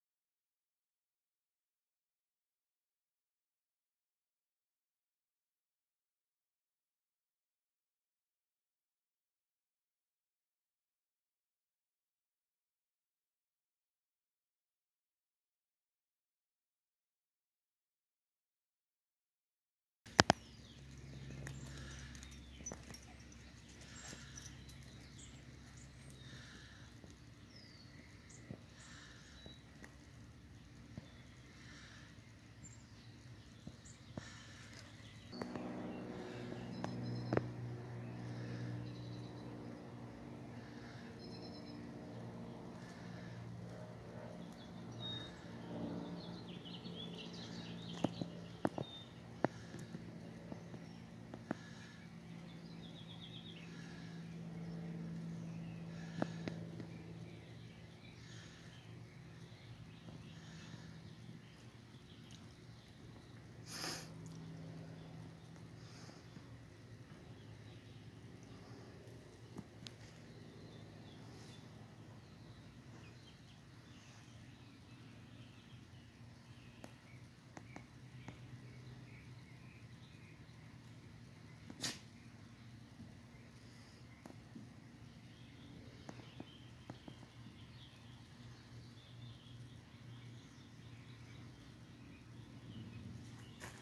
MONARCH WITH CREDITS VIDEO
We are serving as surrogates to the Monarch caterpillars we find on the milkweed in our back yard. We gather them, along with the leaves they are munching on, and bring them into the house to be placed in a butterfly cage. This is a video of a female Monarch butterfly flitting from stalk to stalk laying eggs as she goes. We gather them, place them in the netted butterfly cage (the Monarch Motel) where they are safe, and then watch as they evolve into pupas and them into the beautiful Monarchs they are destined to be. Credit to Marilyn Brooks for heading the Monarch Project of Granada Hills.